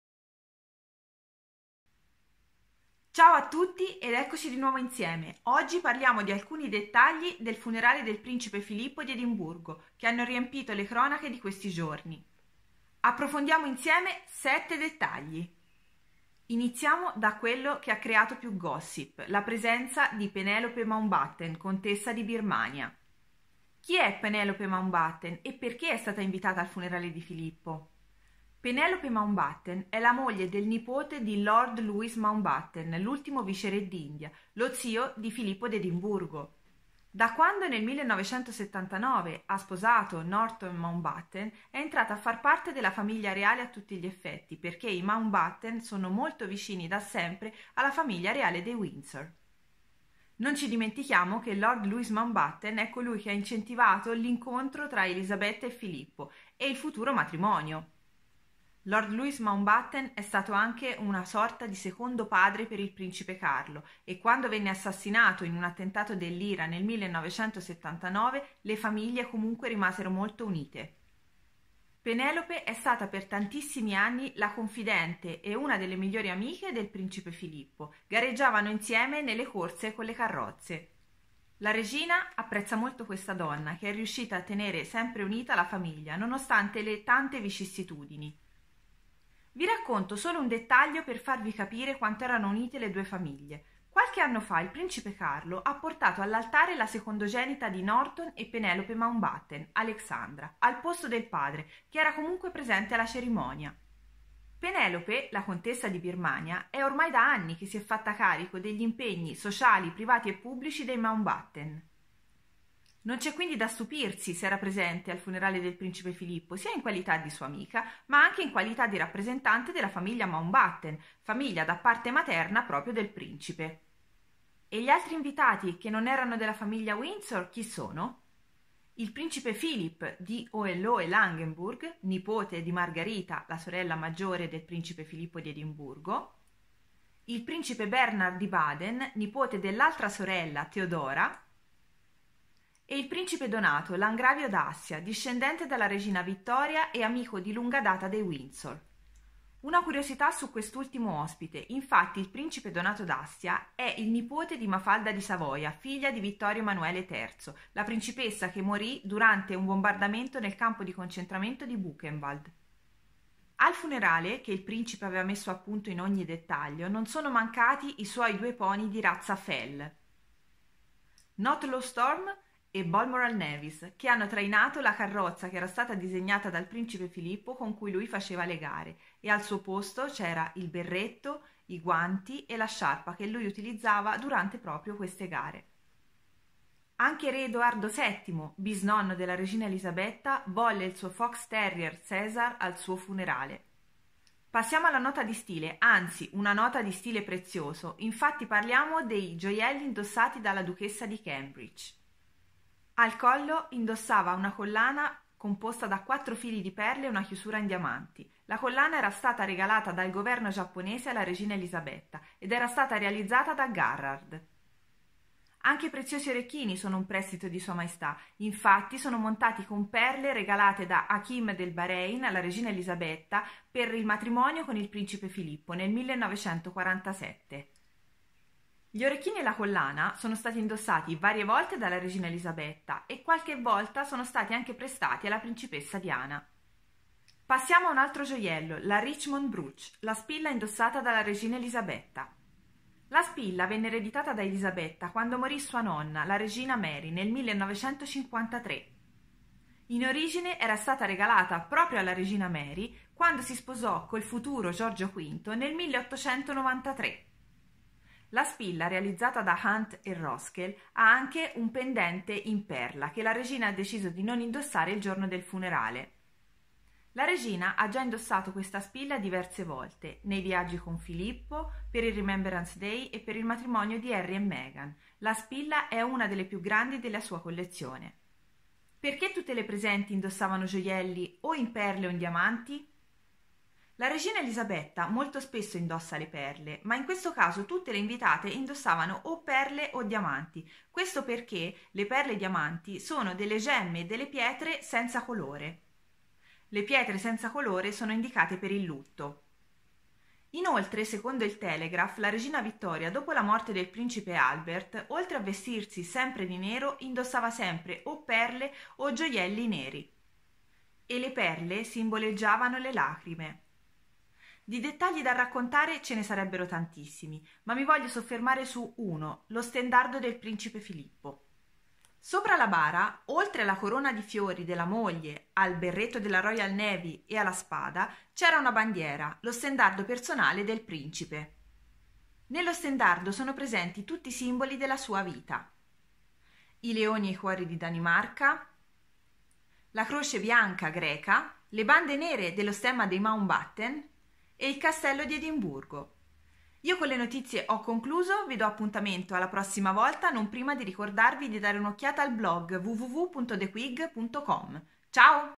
Il Funerale del Principe Filippo - 7 dettagli
7 dettagli sul funerale del Principe Filippo, dal significato dei gioielli allo stendardo personale, passando per la ormai "famosa" Penelope Mountbatten.